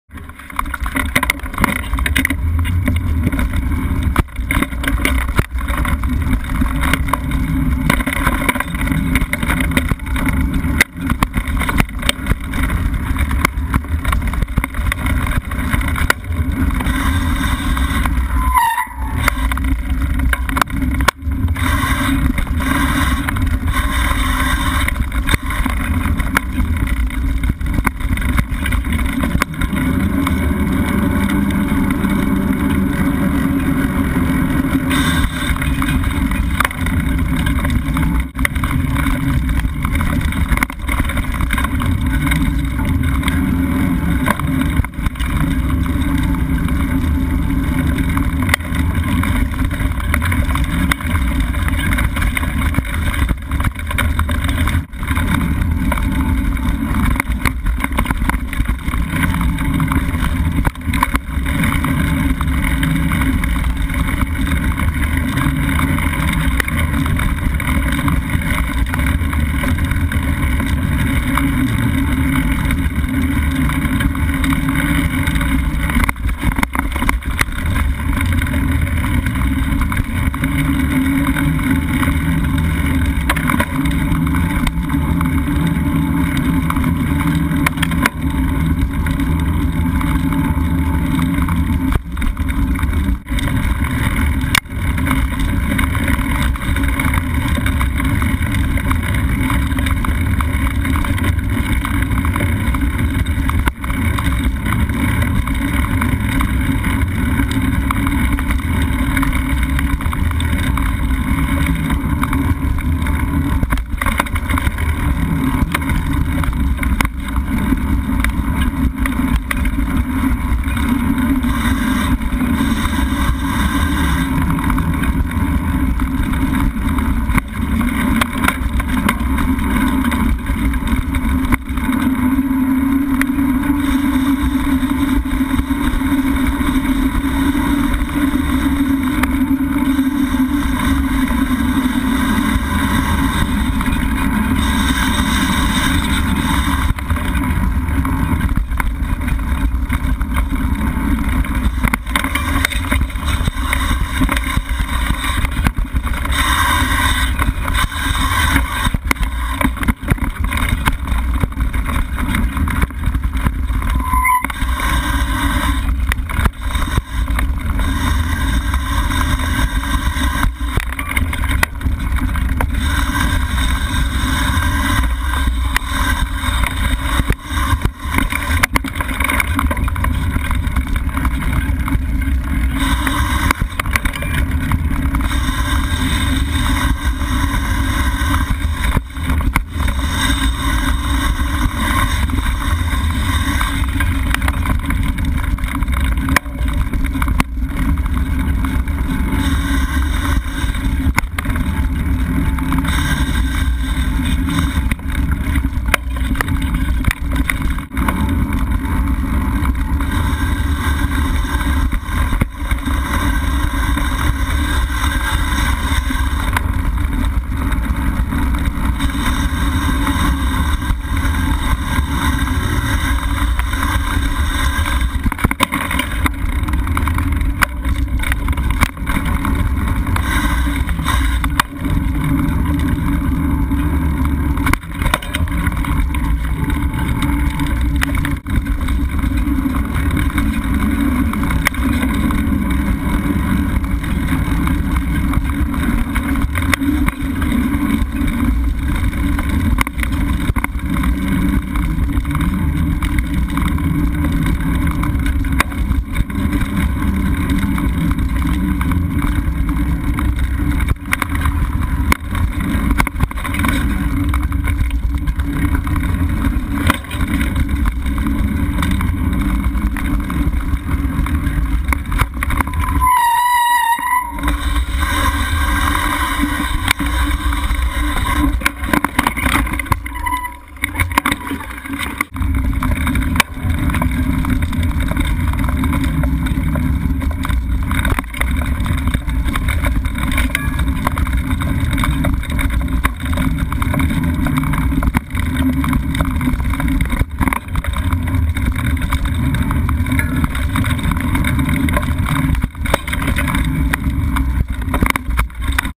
David English 4-23
Short clip of my 20 minute loop on the David English Trail in White Clay Creek State Park, Delaware. With a nice pitiful fall towards the end (still getting used to the whole clipless pedal thing).